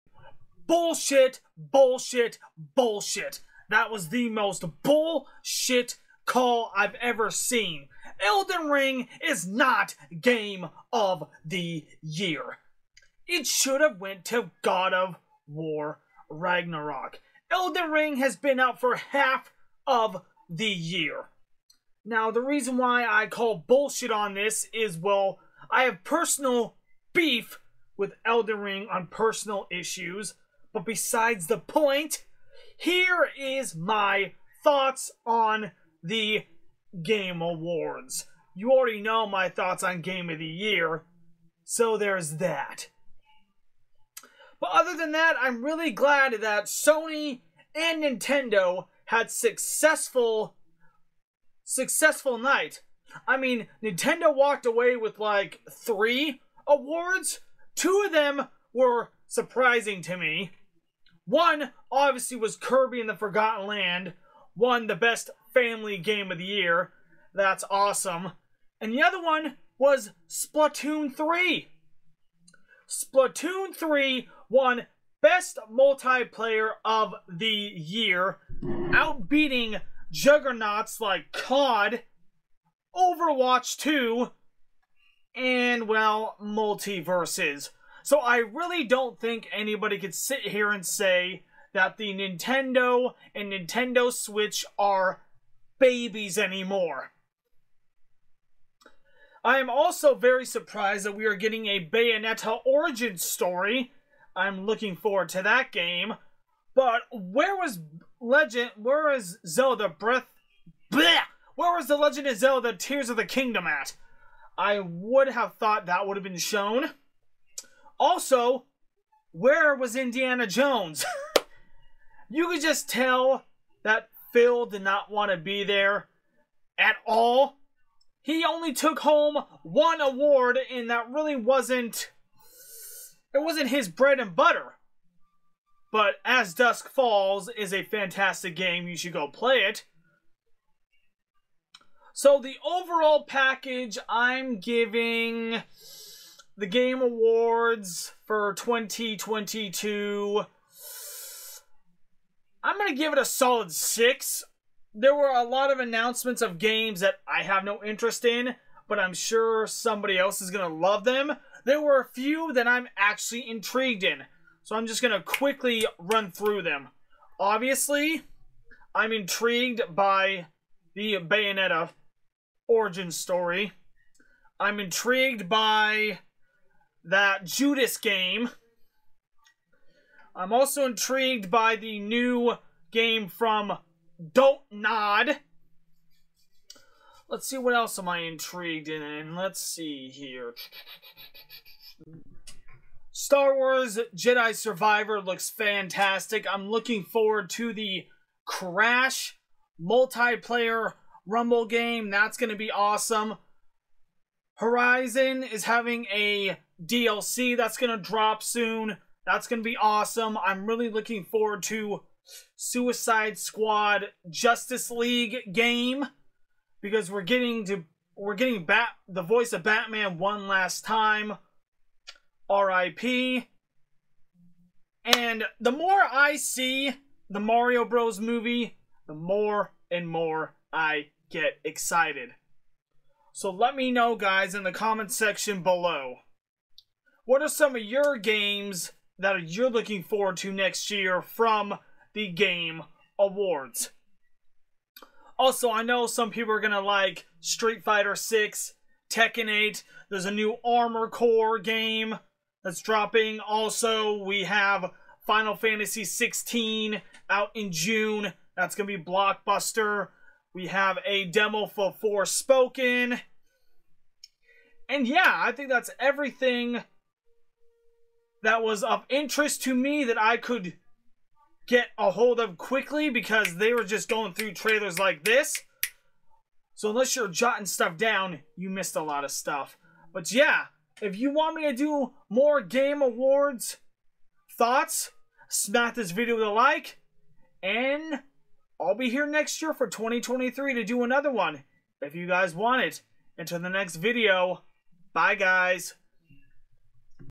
God of war got robbed! The game awards thoughts!
here is my thoughts on the game awards! I hope u enjoy it!